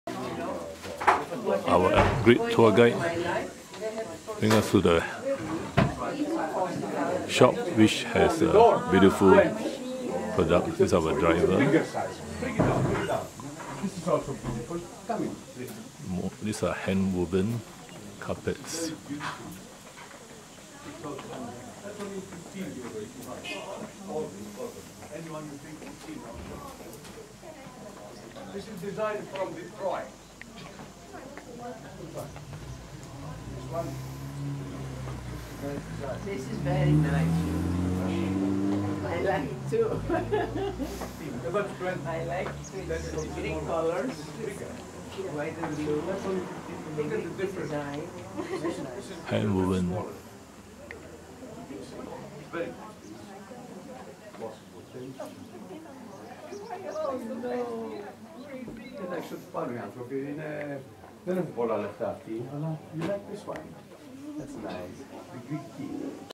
Hill top village, near ski resort. Souvenir shop
Few buyers. A large variety of handwoven carpets.  I bought a small blue handwoven carpet with hill top house design for 18 euros. I suspect they are machine-done as I saw a similar one later in Santorini